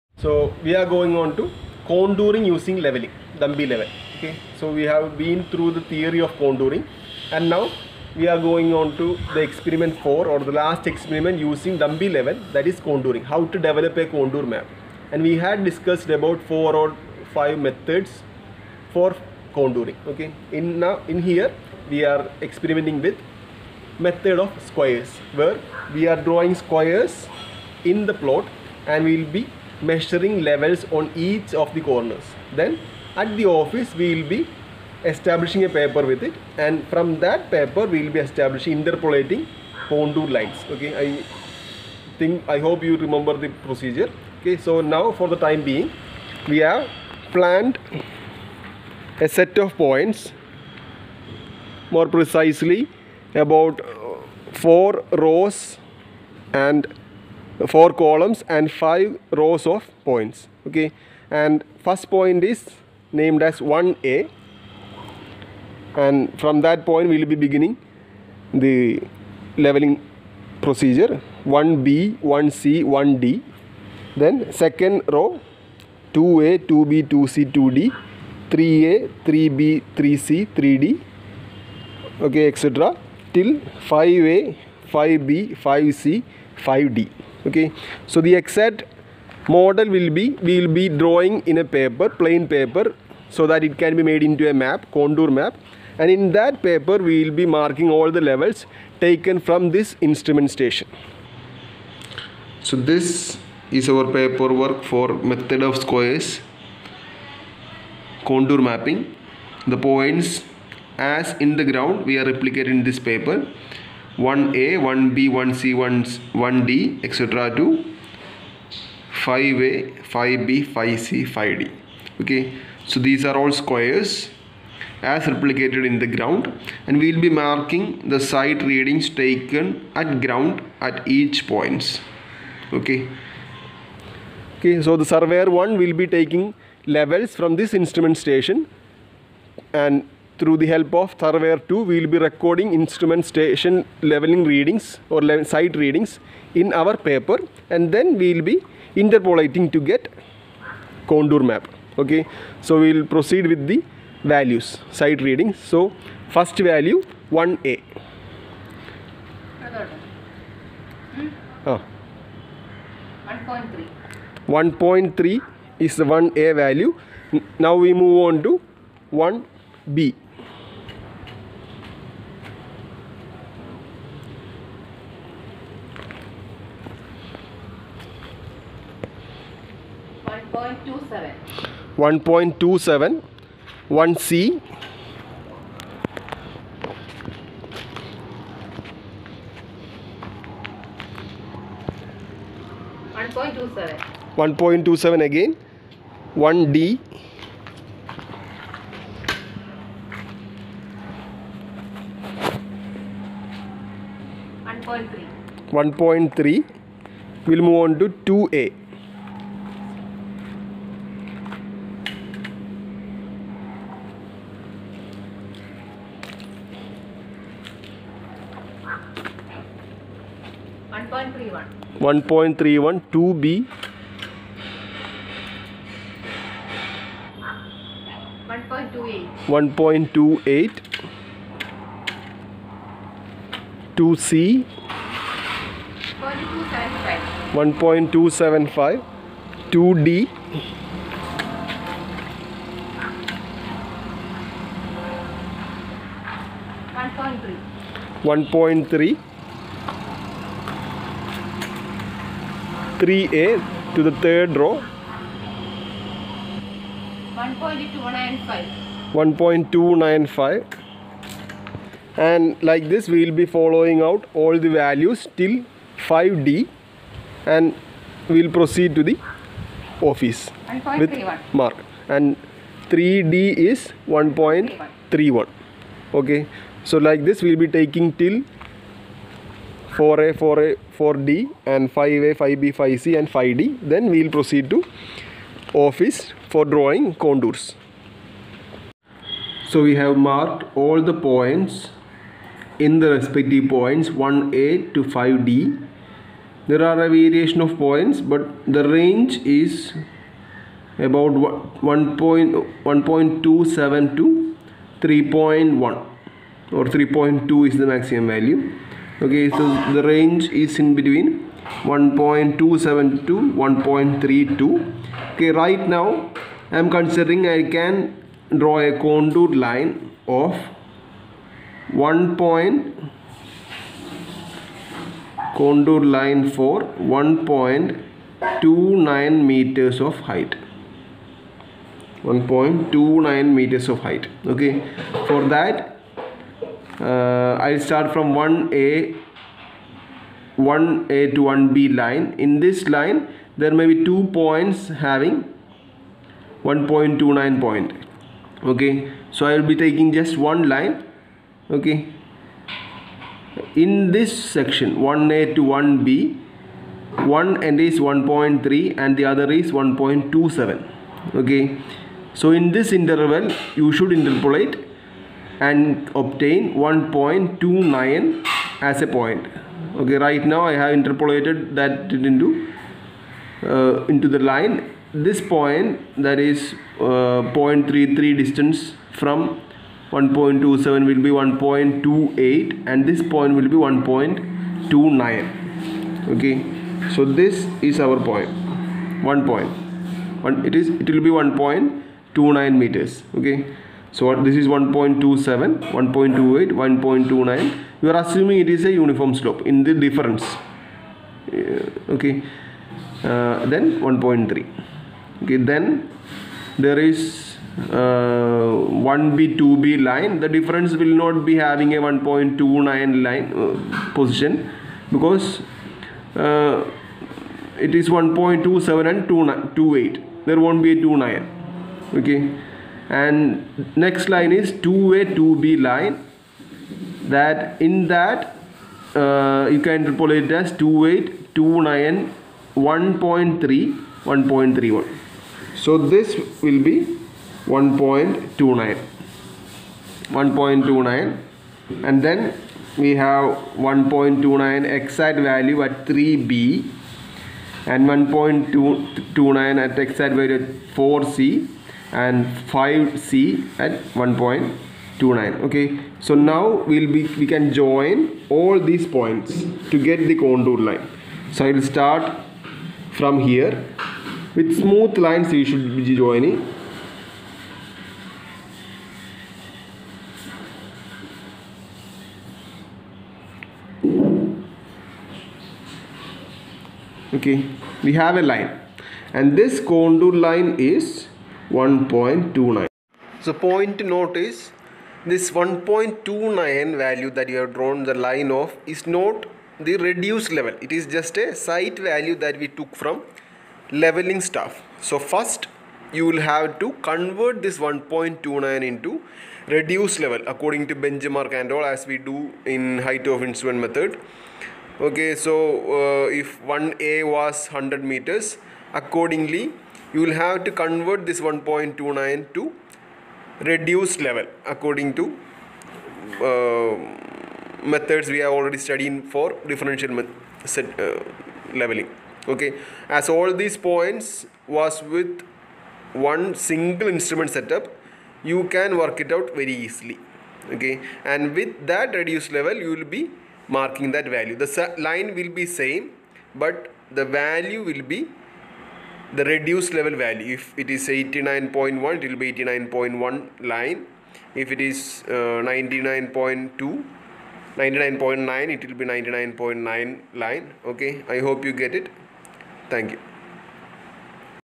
Contouring using Dumpy level : Surveying Practical
Welcome to our practical guide on Contouring Using a Dumpy Level! 📐

In this educational video, we'll take you step-by-step through the process of using a Dumpy Level for contouring, a fundamental skill for surveyors, civil engineers, and anyone interested in land analysis and design.